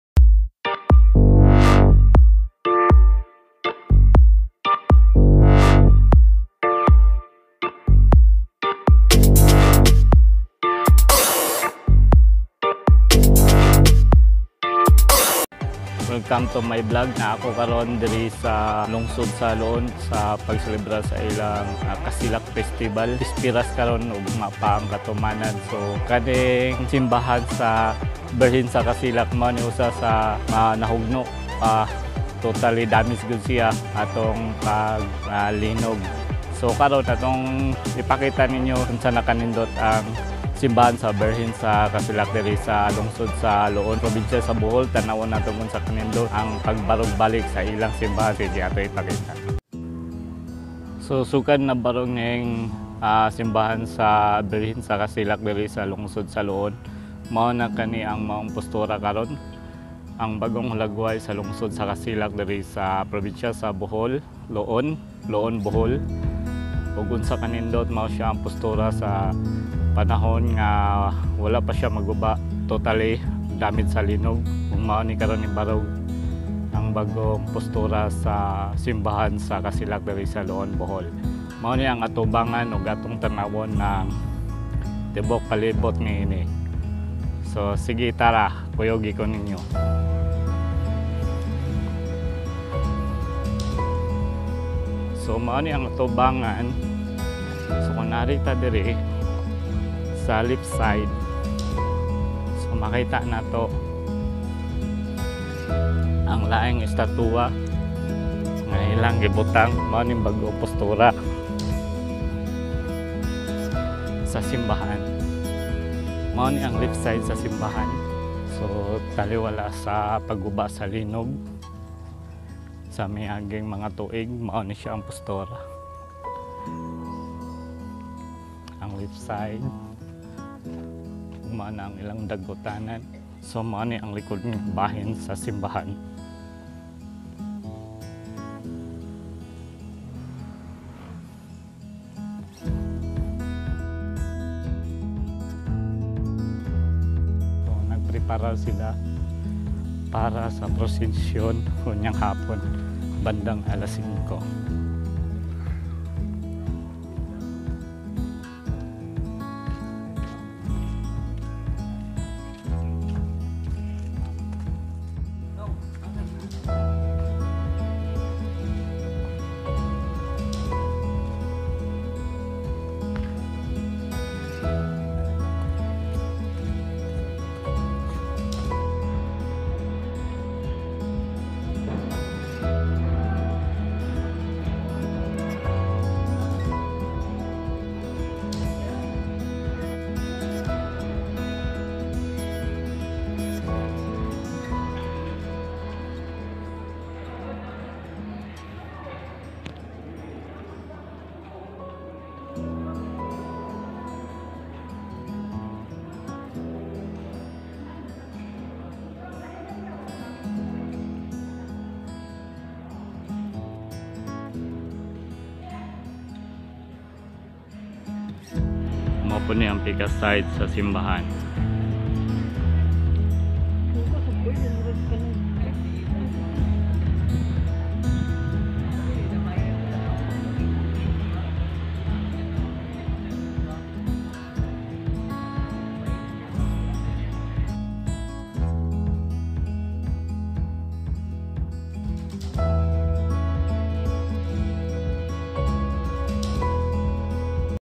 KASILAK (OUR LADY OF LIGHTS CHURCH ANG MULING PAGBANGON)
Our Lady of Lights Church  ay isa ito sa mga maraming simbahan sa probinsya sa Bohol na pinabagsak ng malakas na lindol na tumama noon sa probinsya. Kung gaano ito kaganda noong una ito itinayo mas lalong pa itong pinaganda sa muling pagbangon at pinagbase pa rin ito sa dati nitong disinyo. Tara ating tuklasin.